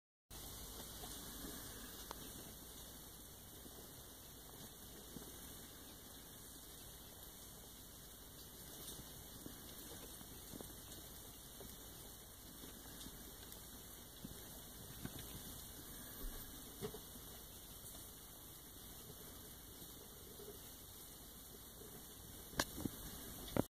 Little Kenyan Roaches - Blaberidae sp. Kenya
A look at some Little Kenyan roaches waiting for new leaves to hide in. This is the smallest roach currently in the hobby. Excellent feeder for dart frogs, small spiders, geckos, chams, mantids and other small insectivores.